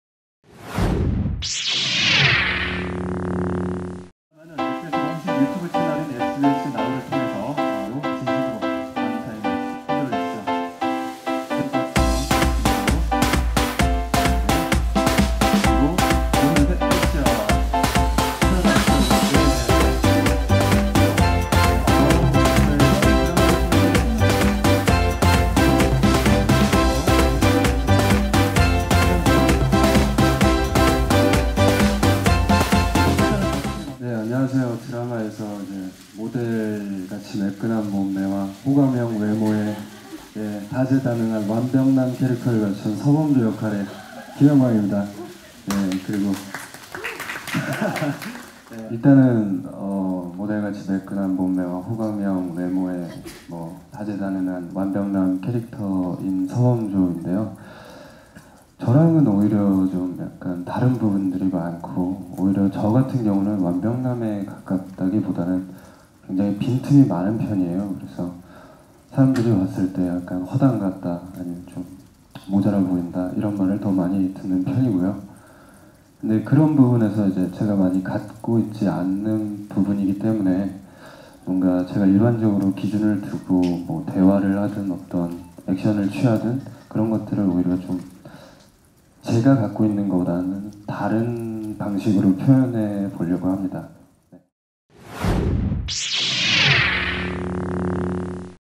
[SSTV] ‘피노키오’ 김영광 “실제 모습 완벽남? 빈틈 많고 허당”
★All Kpop photos SSTV 포토★

[SSTV l 조성욱 기자] SBS 새 수목드라마 ‘피노키오’(연출 조수원 신승우 l 극본 박혜련) 제작발표회가 6일 오후 서울 목동 SBS에서 열렸다. 이날 제작발표회에는 이종석 박신혜 김영광 이유비 이필모가 참석했다.